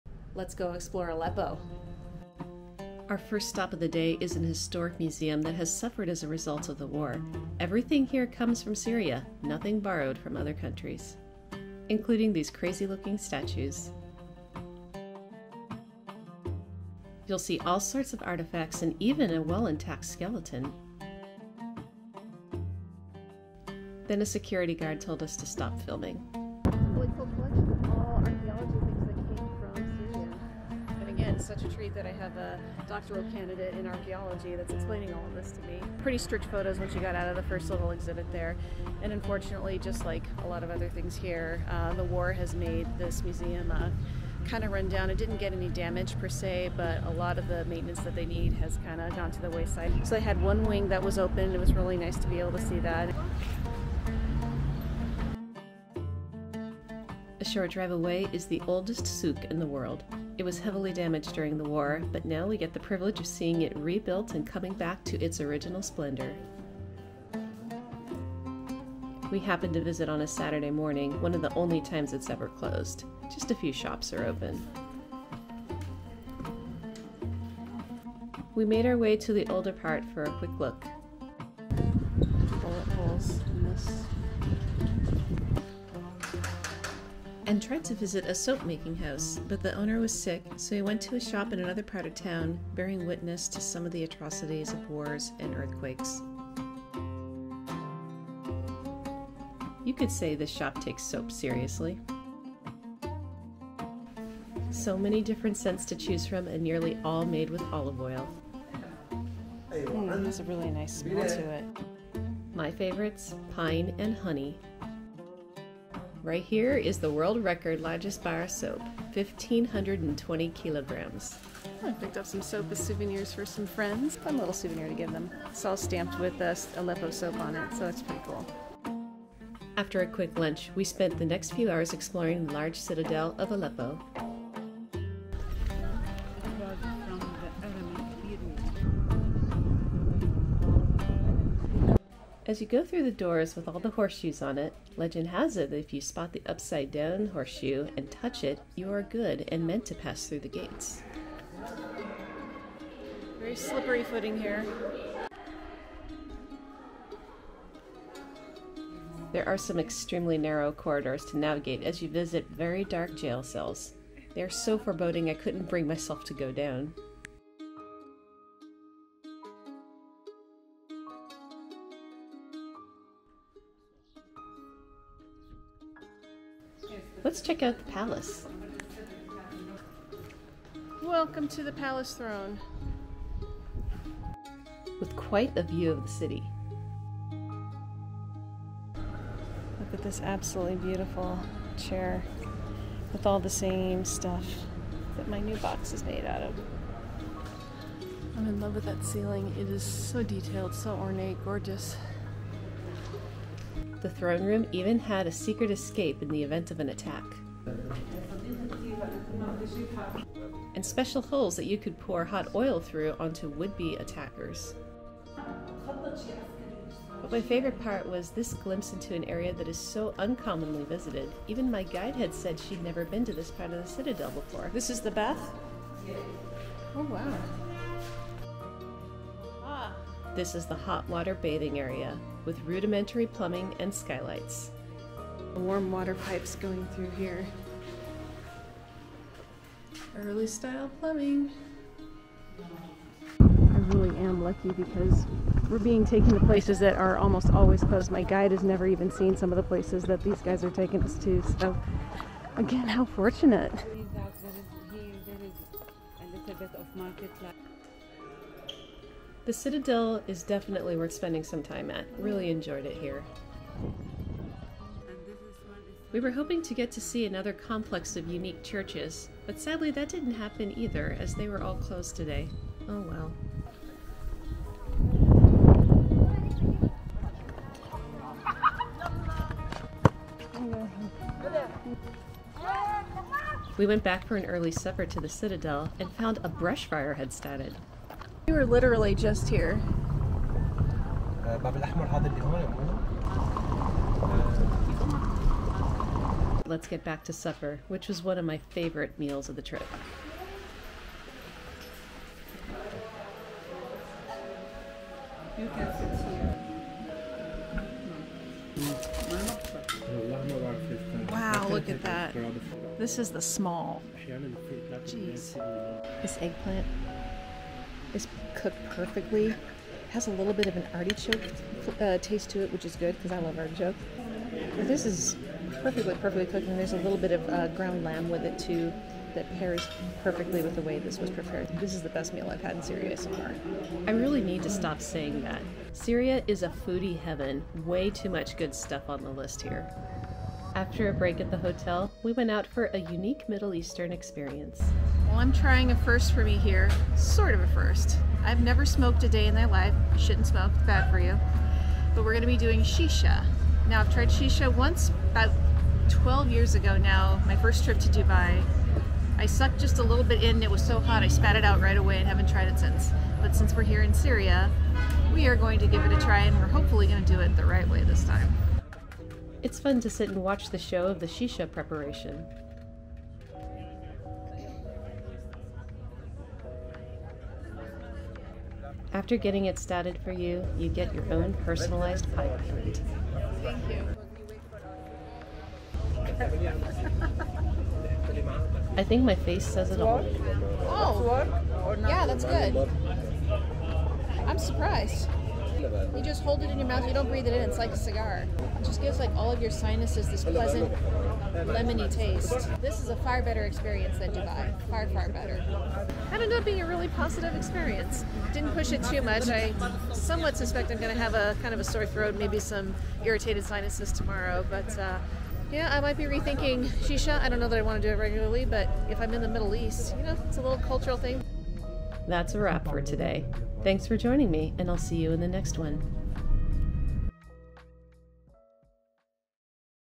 Day 4 As A Tourist In Syria...First Time Trying Shisha...Exploring Aleppo
This was filmed in spring of 2024. What another great day of learning about their history, amazing foods...and a first for me...shisha! And as it turns out...with the right flavor...it's not so bad! Hope you enjoy this glimpse into what life as a tourist in Syria is like!